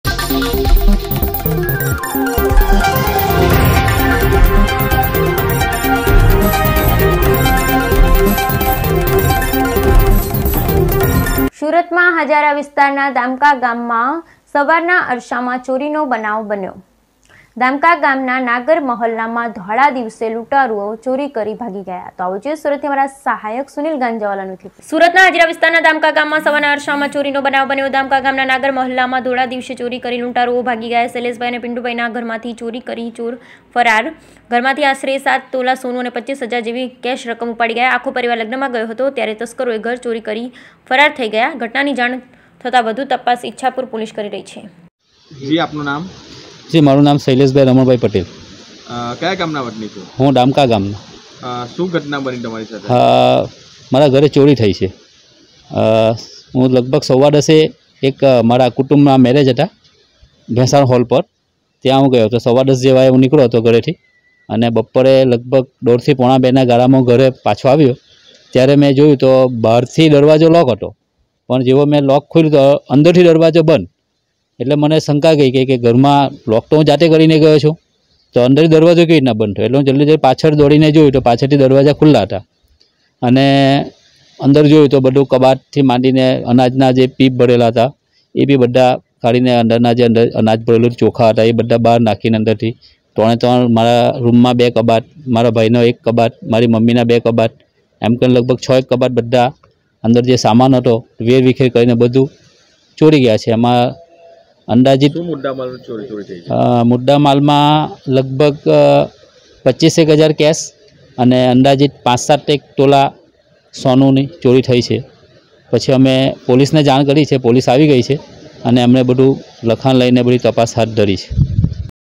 SURAT:-સુરતના હઝીરા વિસ્તારના દામકા ગામના નાગર મોહલ્લા માં ધોરા દિવસ એ ચોરી કરી લૂંટારા ઓ ભાગી ગયા
સુરત ના હઝીરા વિસ્તાર ના દામકા ગામ સવાર ના અર્ષમાં ચોરી નો બનાવ બન્યો

દામકા ગામ ના નાગર મોહલ્લા માં ધોરા દિવસ એ ચોરી કરી લૂંટારા ઓ ભાગી ગયા

શૈલેષભાઇ અને પીન્ટુભાઇ ના ના ઘર માં થી ચોર ચોરી કરી ફરાર

ઘર માં થી આશરે 7 તોલા સોનુ અને 25000 જેવી કેશ રકમ ઉપાડી ગયા

આખો પરિવાર લગ્ન માં ગયો હતો ત્યારે તસ્કારો એ ઘર ચોરી કરી ફરાર થઈ ગયા

ઘટના ની જાણ થતા વધુ તપાસ ઈચ્છાપુર પોલીસ કરી રહી છે